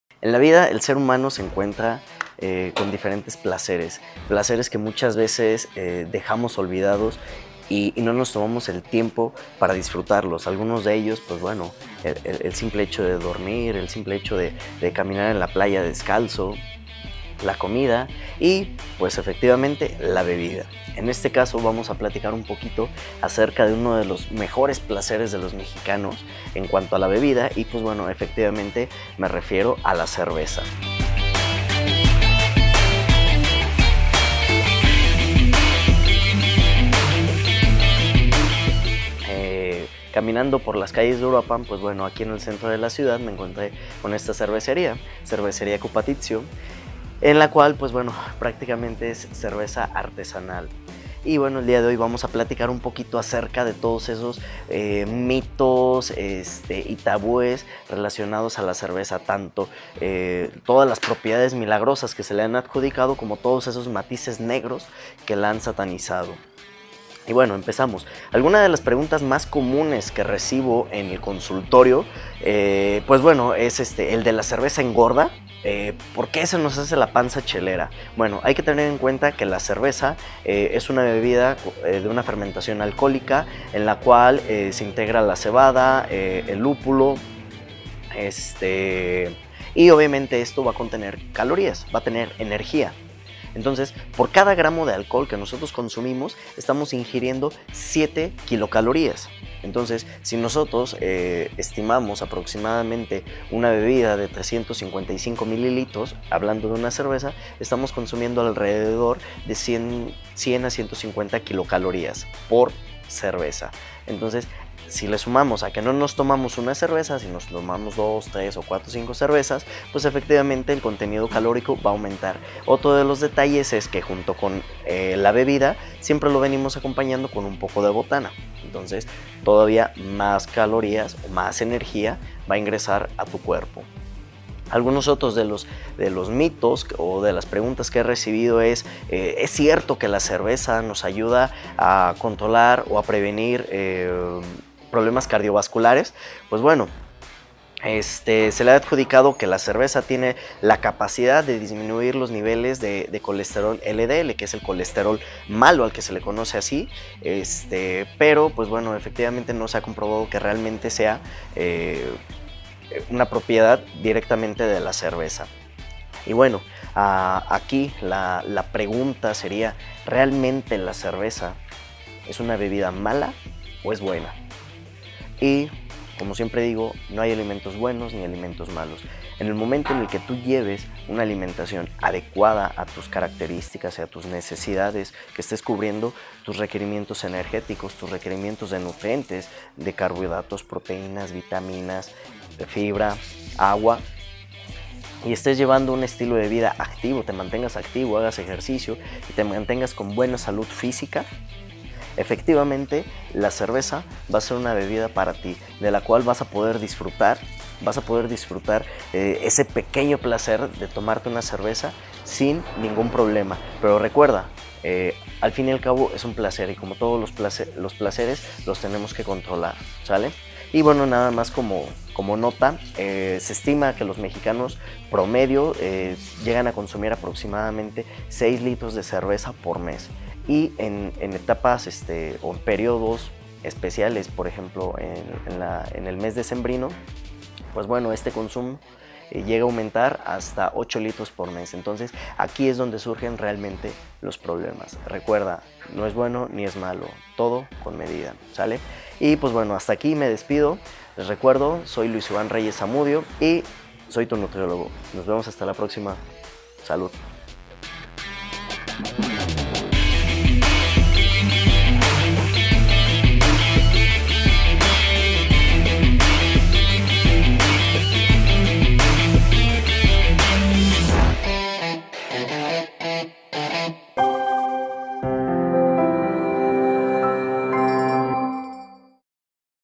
LA CERVEZA ¿BUENA O MALA?
El nutriólogo Luis Ivan Reyes nos comparte qué tanto daño o beneificio nos puede causar el consumo de cerveza. Checa esos datos.